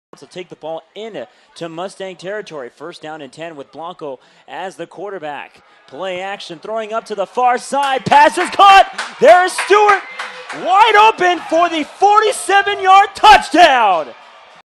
47 Yard Passing Touchdown from #5 Fernando Blanco to #1 Michael Stewart of Tahquitz
The Tahquitz Titans put up another score from a 47 yard pass from Fernando Blanco to Michael Stewart against the West Valley Mustangs in this CIF Central Coast Section match up.